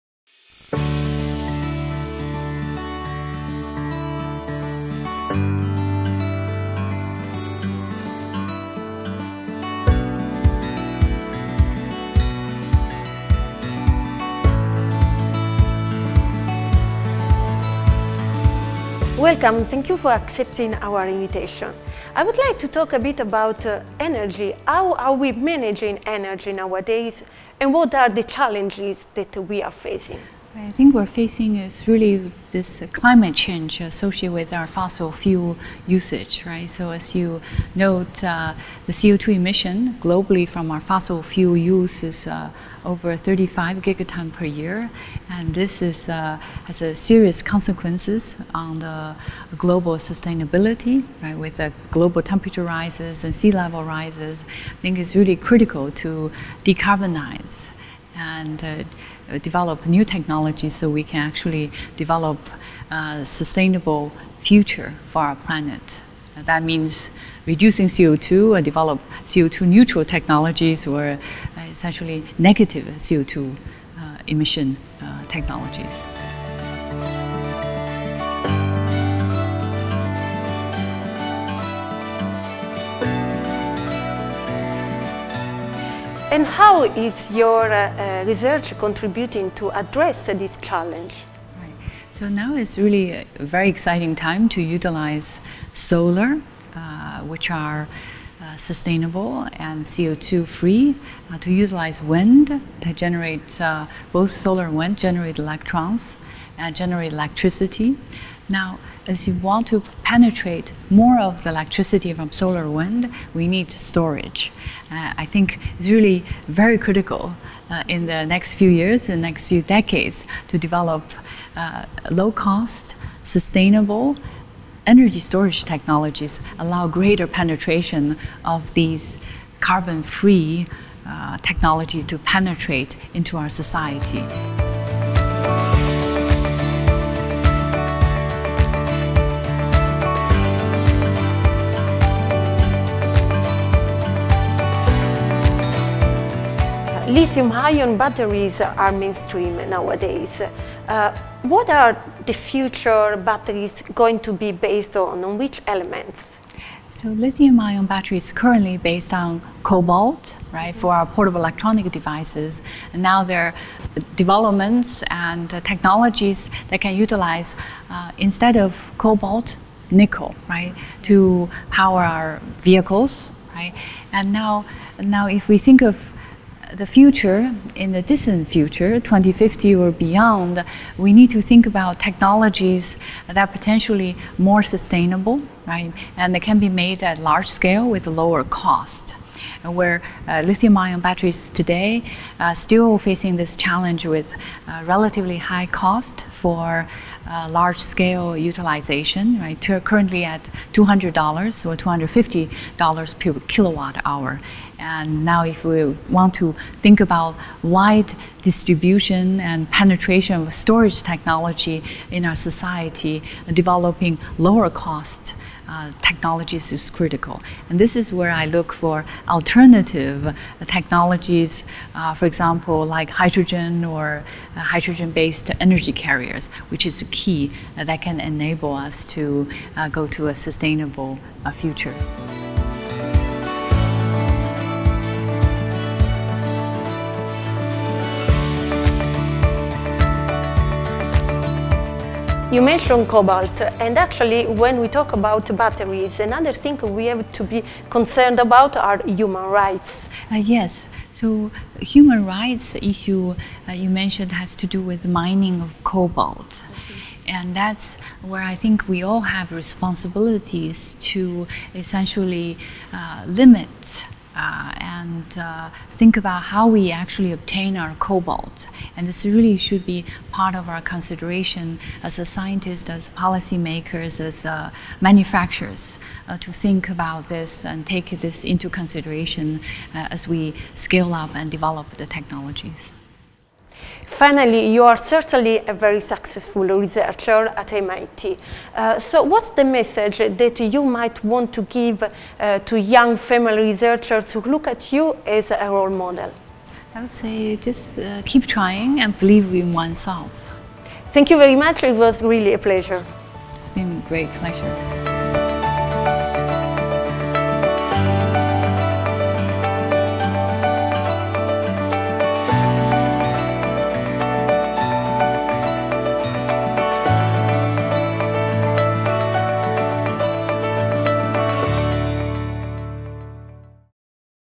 New energy storage solutions to decarbonize our society – Interview with Prof. Yang Shao-Horn
Prof. Yang Shao-Horn, Professor of Energy at the Massachusetts Institute of Technology and one of The World’s Most Influential Scientific Minds, according to Thomson Reuters, was invited to give a talk on electrochemical energy storage on June 17 2019 at the ICN2 as part of the Manuel Cardona Lecture series.

In this interview performed almost a year ago she talks about the challenges that we are facing in terms of energy sustainability, resource consumption and efficiency of our storage systems. Prof. Shao-Horn also discusses the importance of developing alternative ways to generate and manage energy with the aim of decarbonizing our society.

Her talk and this interview were developed in the context of the ICN2 Manuel Cardona Lectures, named as a tribute to Prof. Manuel Cardona and established to bring prominent researchers in nano-related fields to the Barcelona area to share their findings and vision with researchers from the local academic and industrial ecosystems.